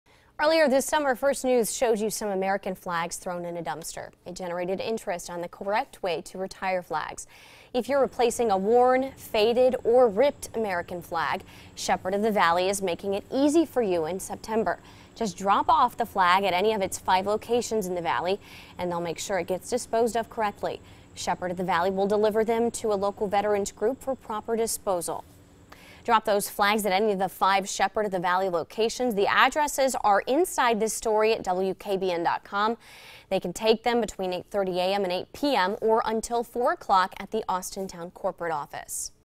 Shepherd of the Valley disposing of old American flags
If you're replacing a worn, faded, or ripped American flag, Shepherd of the Valley is making it easy for you to retire the old flag.